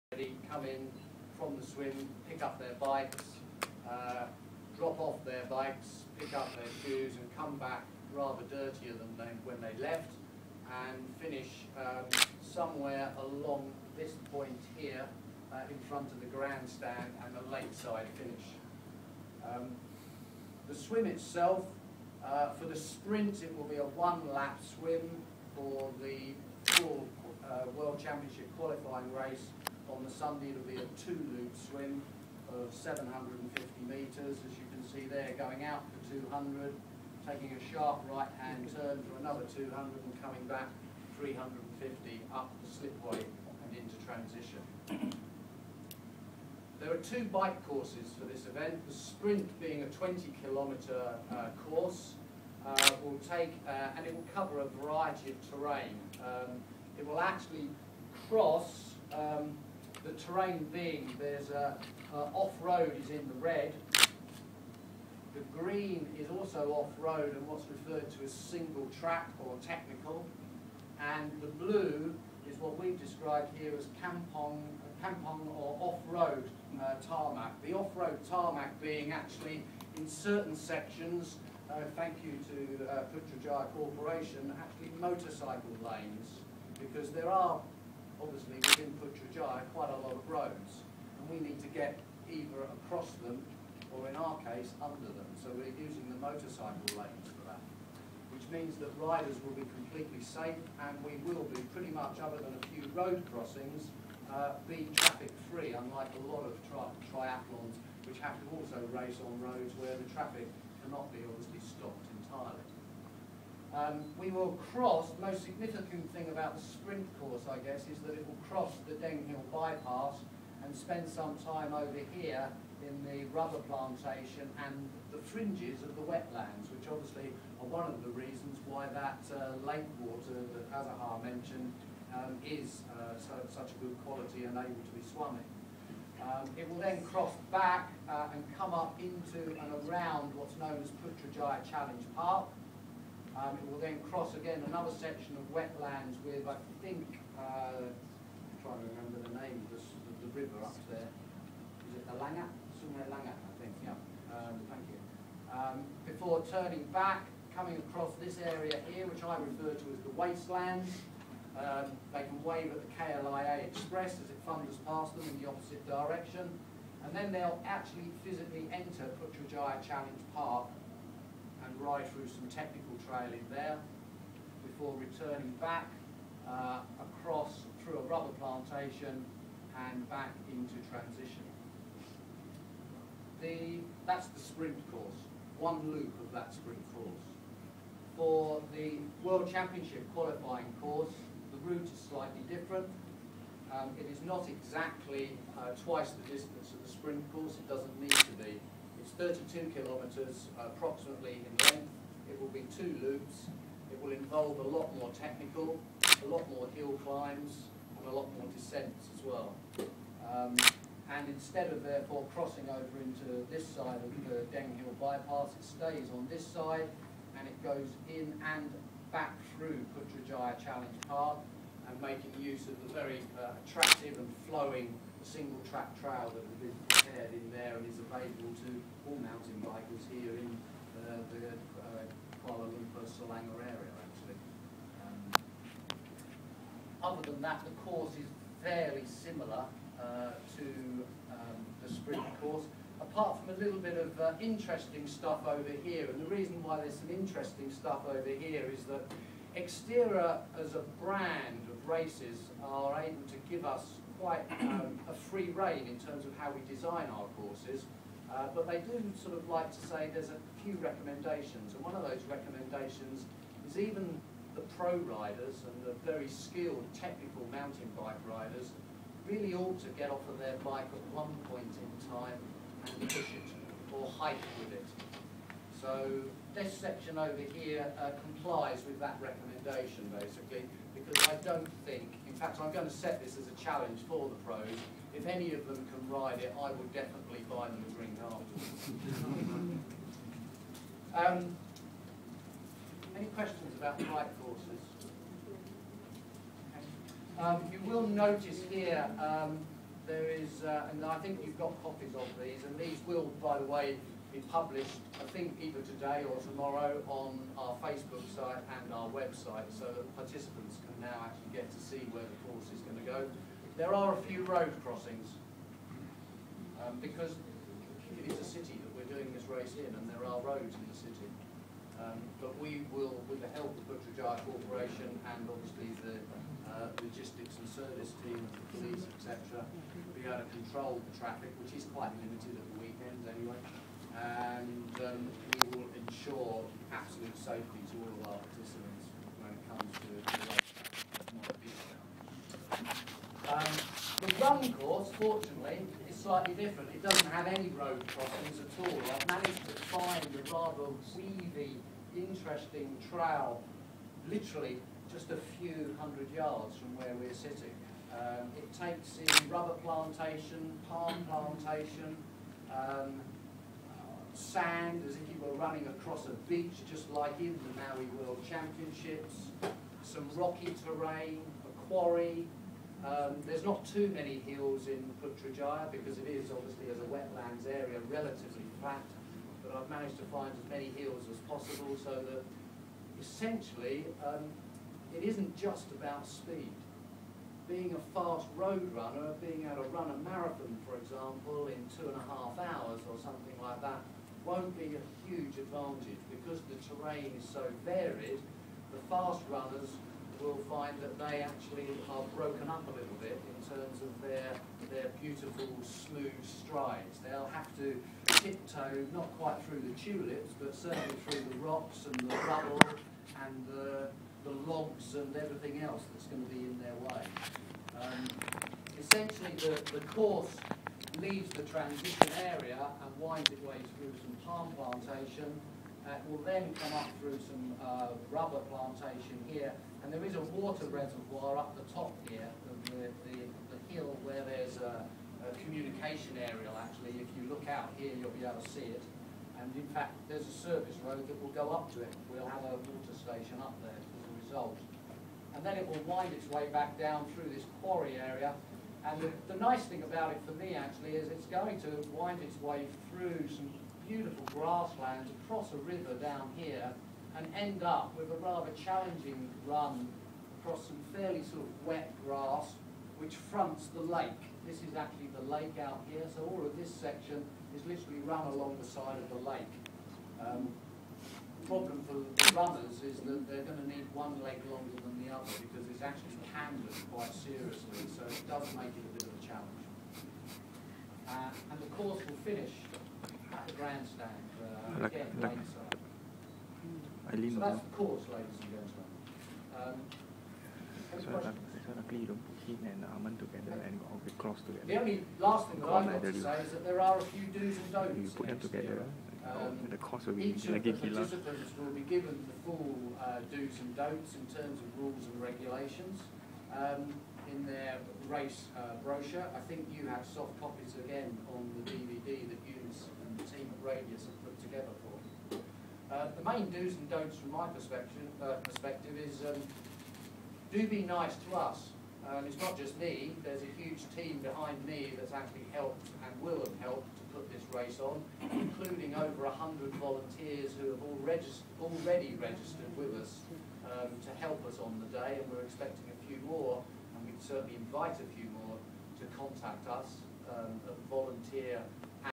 Xterra Malaysia Race Director Course Preview
Dave Spence introducing the race route for Xterra Malaysia 2013